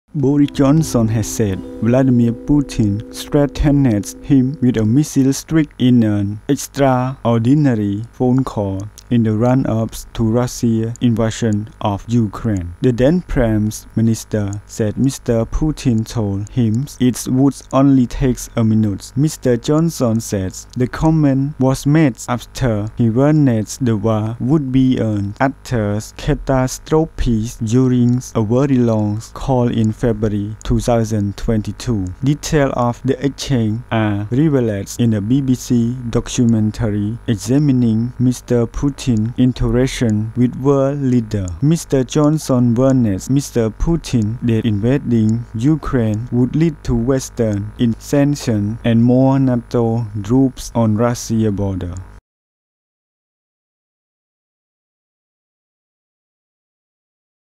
Ukraine Boris Johnson says Putin threatened him with missile strike
Boris Johnson has said Vladimir Putin threatened him with a missile strike in an "extraordinary" phone call in the run-up to Russia's invasion of Ukraine.